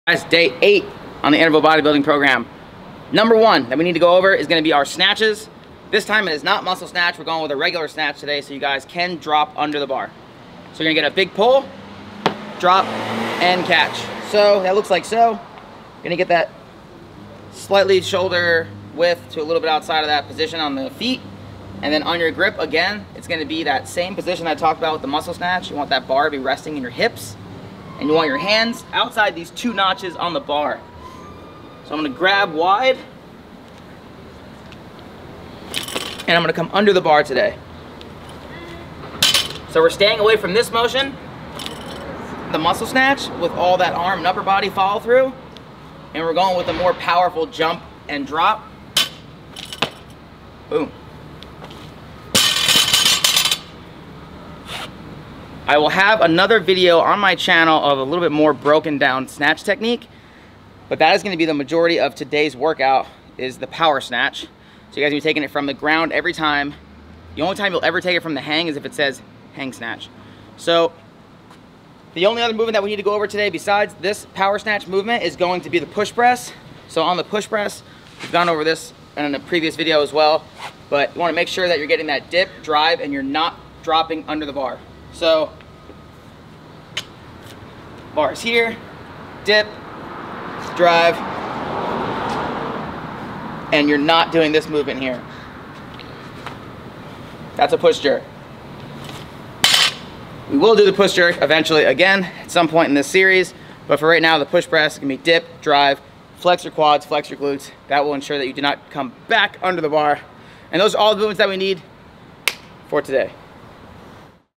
08 Bodybuilding Program
The program follows a 3 day on 1 day off schedule and has exactly 30 days of workouts!
- Every day is very unique and should have no problem keeping your enthusiasm
- You can utilize this program to better yourself as an all-around athlete